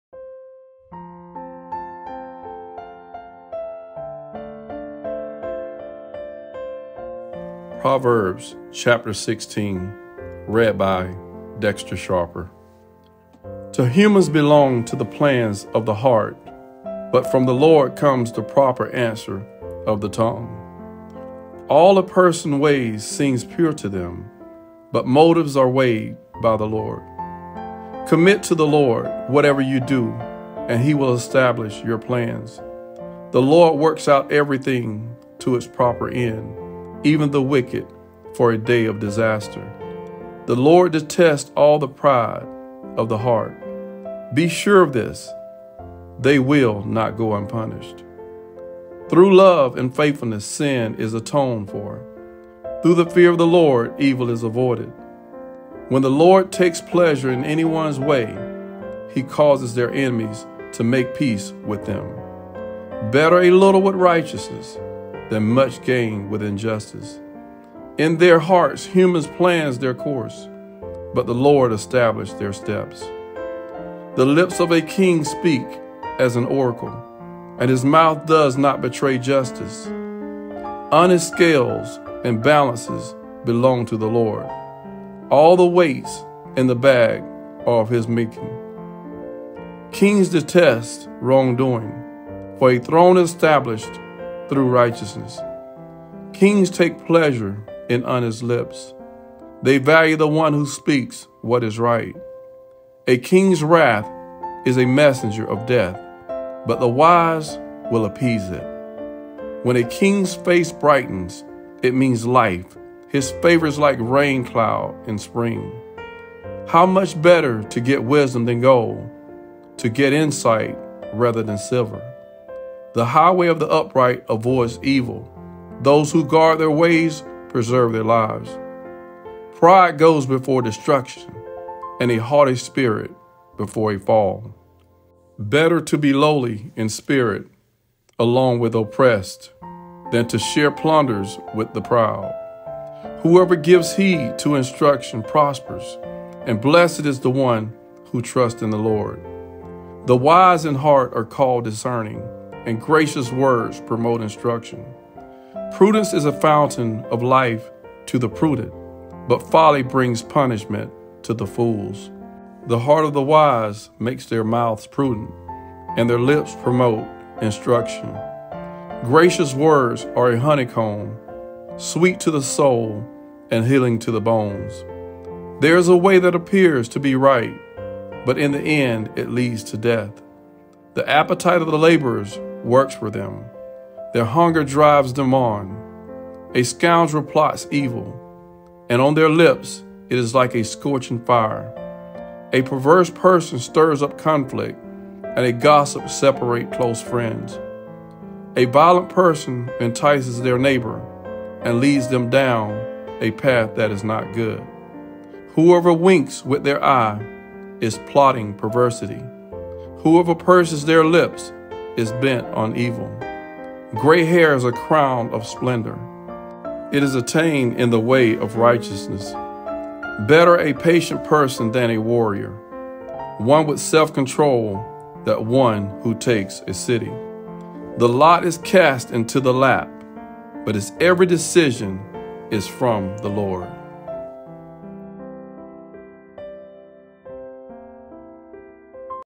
Discovering Timeless Wisdom: Exploring Proverbs 16 read by Dexter Sharper
In this video, we will dive into Proverbs 16 in the Bible, a chapter filled with insightful and practical wisdom for living a fulfilling life. Join us as we explore the themes of trust, humility, wisdom, and planning, and how they apply to our daily lives.

From the importance of seeking guidance from God to the value of honesty and integrity, we will unpack the timeless truths found in this chapter. Whether you are a believer or not, this video will offer valuable insights and practical advice for navigating the complexities of life.

Don't miss out on this opportunity to discover the wisdom of Proverbs 16!

RATE US TODAY USING GOOGLE REVIEW